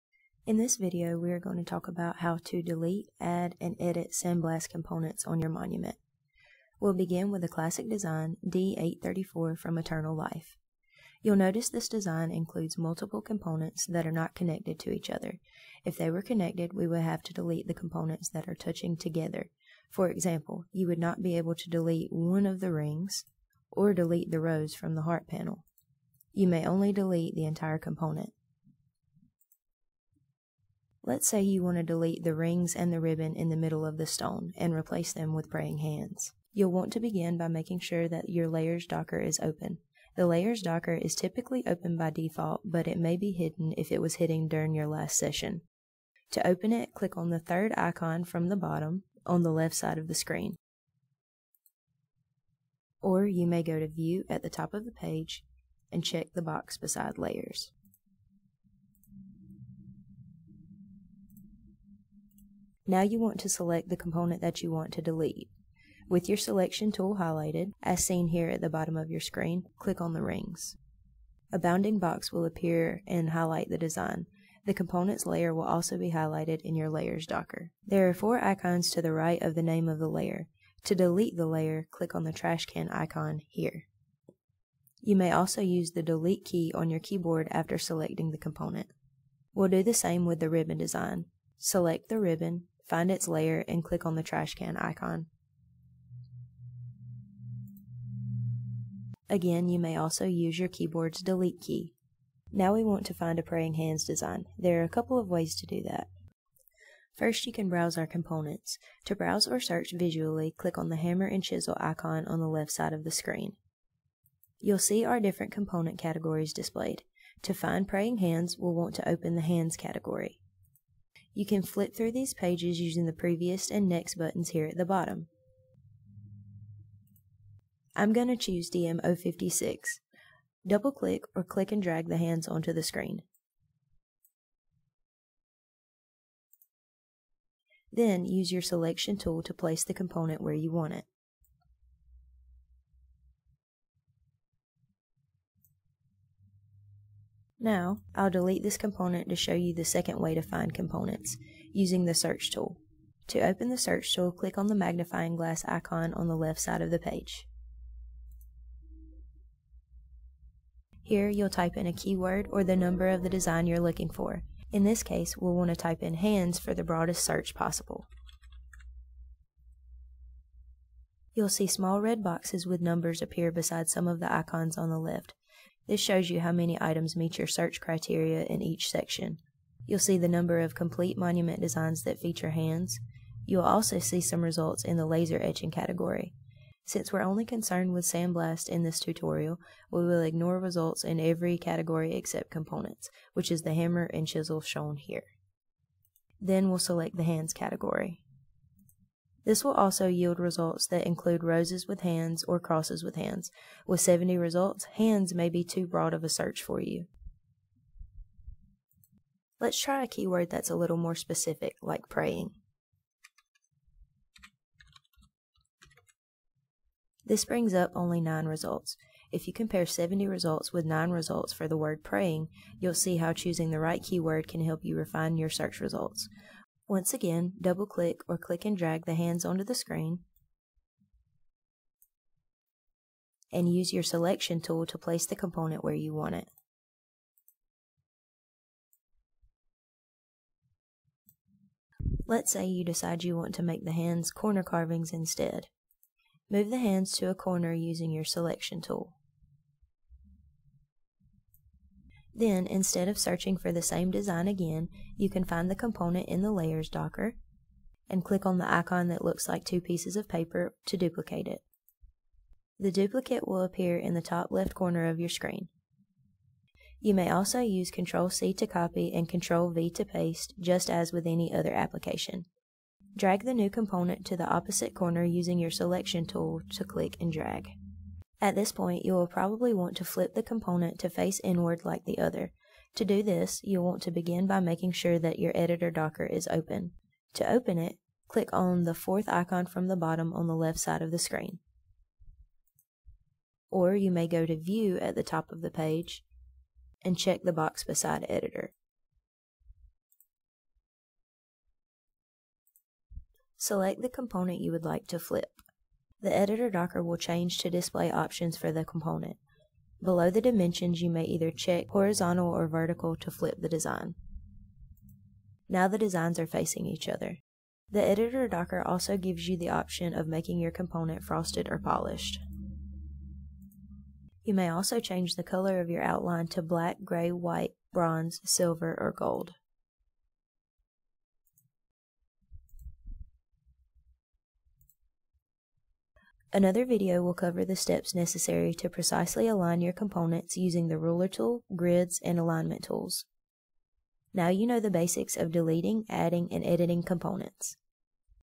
Adding, Deleting, and Editing Components
We'll go over how to delete, add, and edit components you want to use for your monument design.  We can connect to you remotely and share the mouse as we answer your questions.  Other videos will cover the additional capabilities available in the Online Monument Designer.